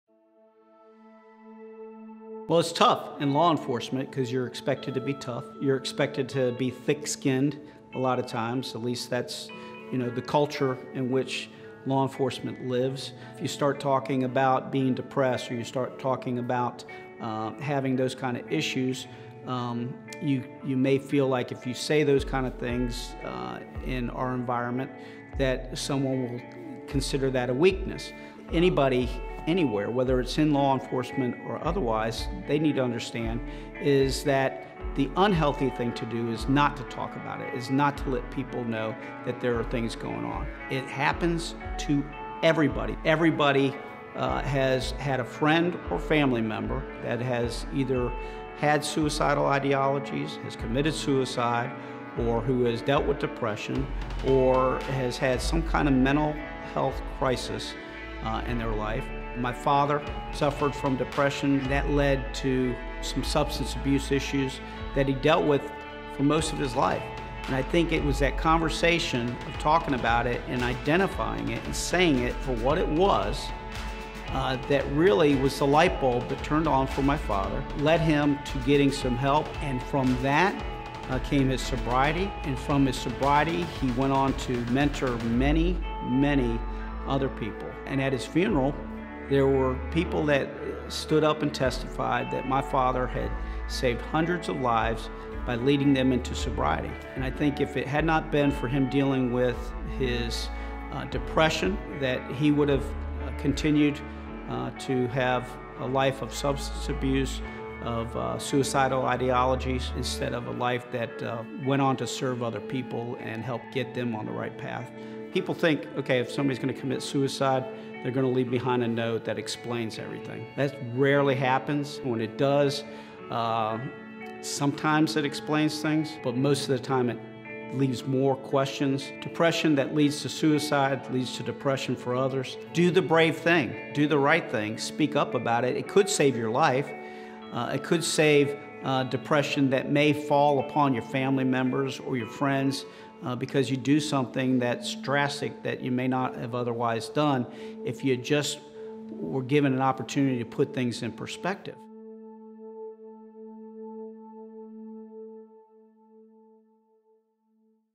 Talk About It Norfolk - Joe Baron
Sheriff Joe Baron reflects on the harsh realities of suicide scenes from his three decades on the job, and how his father’s depression proved to him that talking about it truly can save lives.